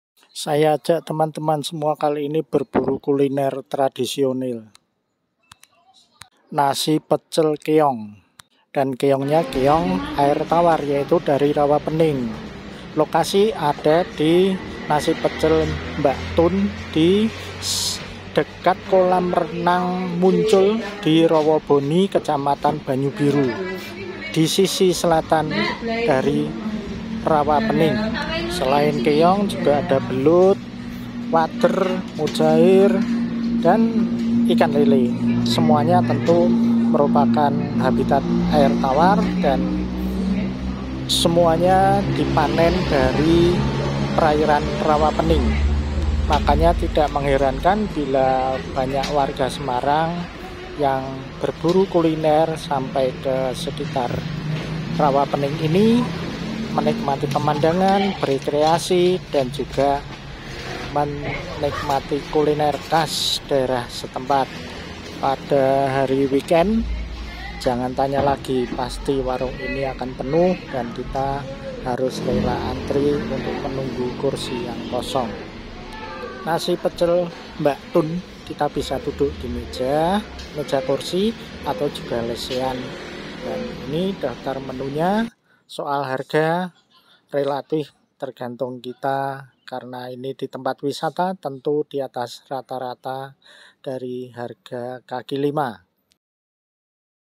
Menikmati Pecel Keong Mbak Toen di Muncul@JChristionoJURAGAN
Nasi pecel Keong Mbak Toen adalah kulinel tradisional yang bisa dinikmati di Muncul, Desa Rowoboni, Kecamatan Banyubiru Kabupaten Semarang. Lokasinya tepat di depan Kolam Renang Muncul.
Keong adalah hasil dari Rawa Pening termasuk juga belum, ikan mujahir dan lele. Semua hasil perikanan di Rawa Pening@JChristionoJURAGAN